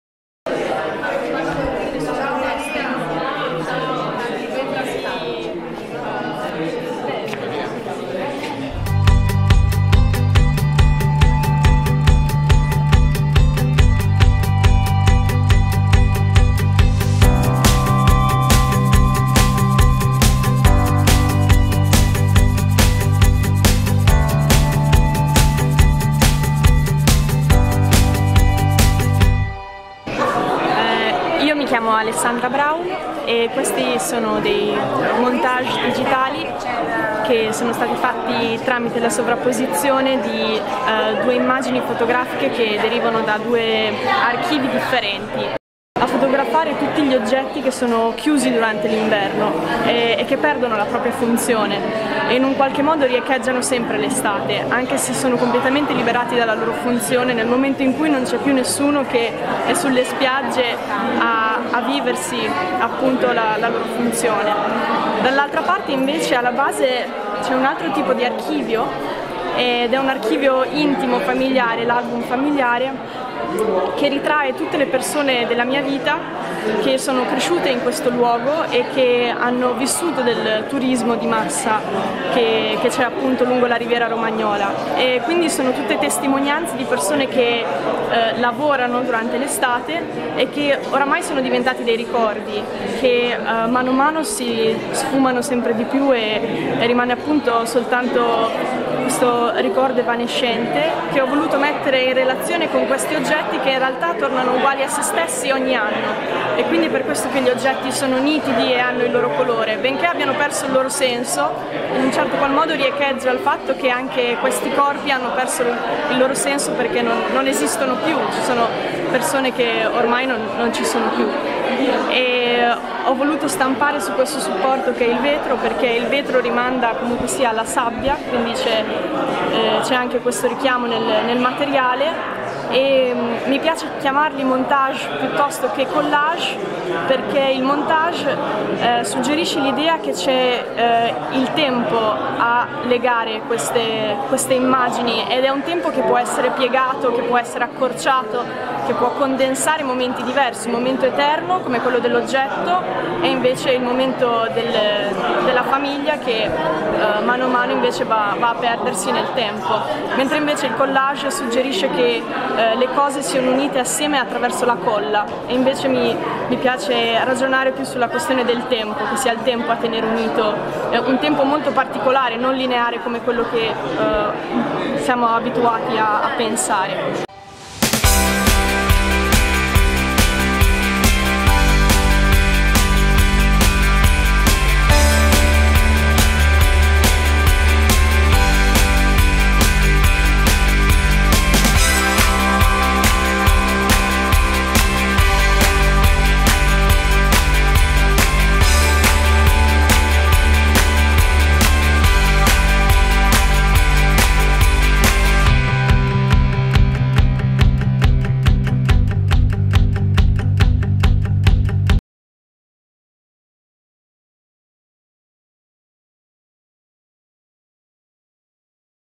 ARCIPELAGO Alessandra Brown
Arcipelago, nessuno è veramente un'isola, è un contest artistico in ricordo di Gustavo Dino Biagi che ogni anno promuove 5 artisti a cui consente di avere il loro primo catalogo ed una mostra all'interno delle sale della Rocca a Bazzano in Valsamoggia. Cominciamo a raccontarvi la prima artista: Alessandra Brown